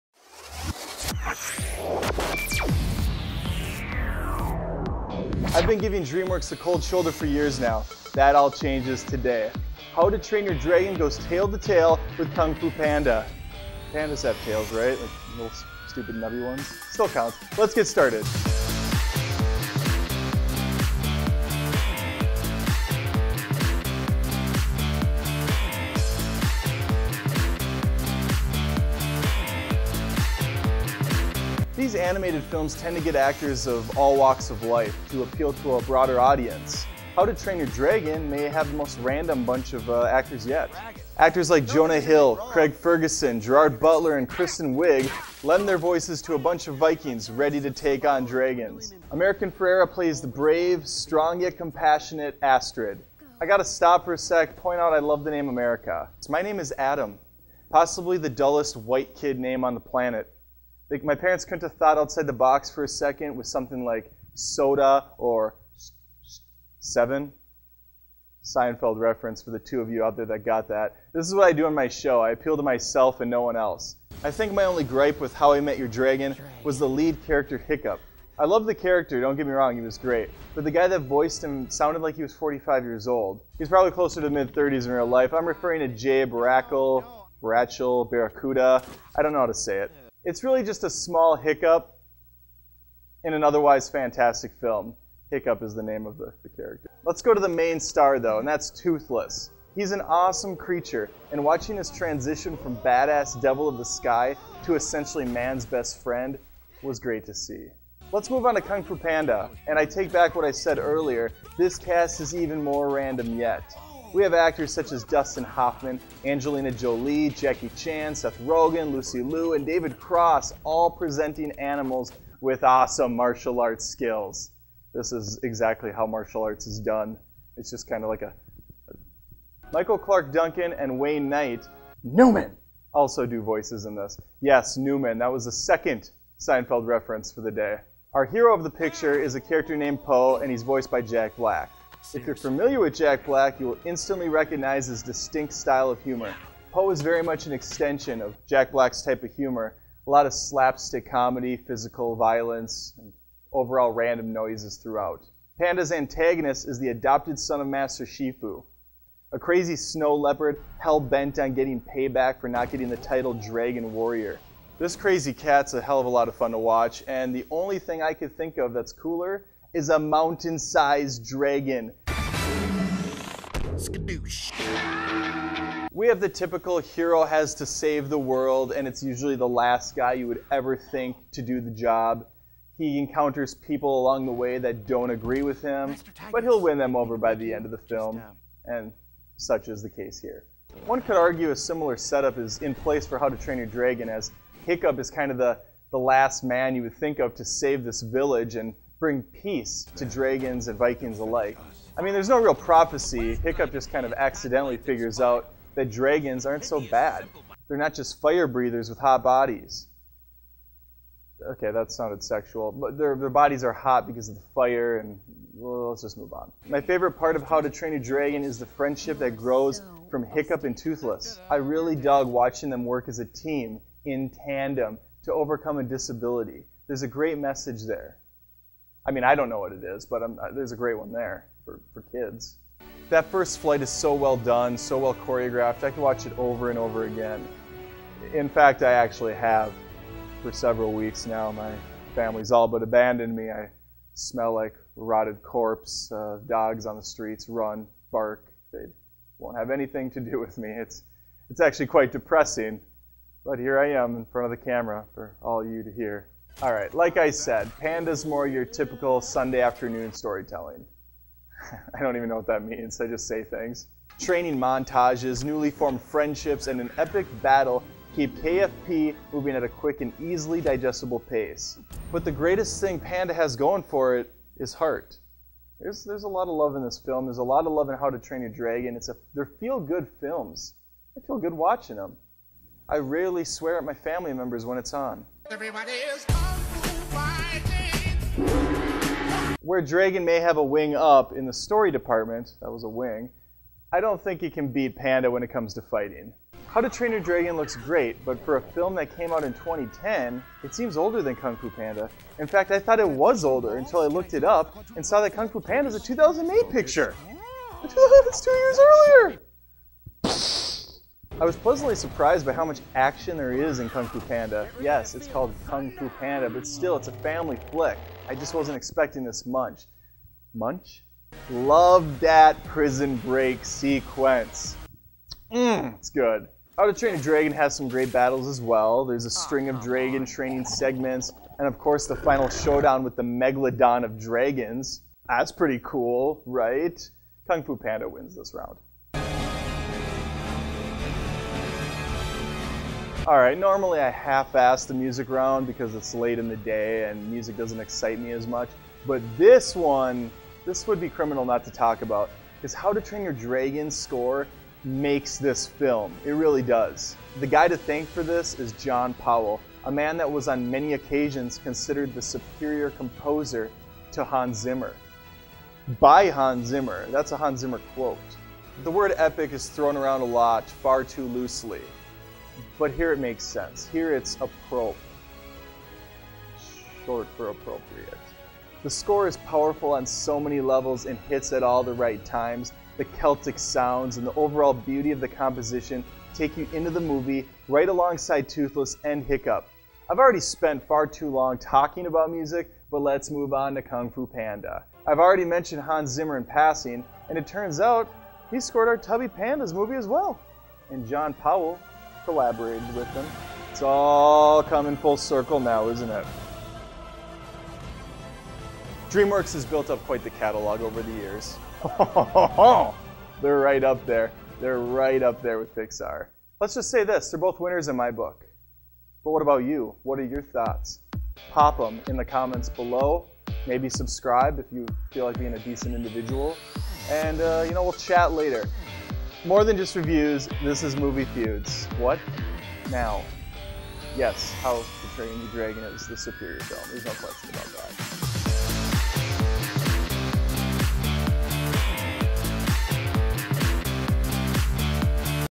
How to Train Your Dragon VS Kung Fu Panda : Movie Feuds ep83
Animals do battle this week as lovable fighting panda Po throws down against the fierce Toothless. This marks the first time 2 Dreamworks films have squared off on Movie Feuds but it certainly won't be the last. Have any other favorites you want to see duke it out? Leave them in the comments below!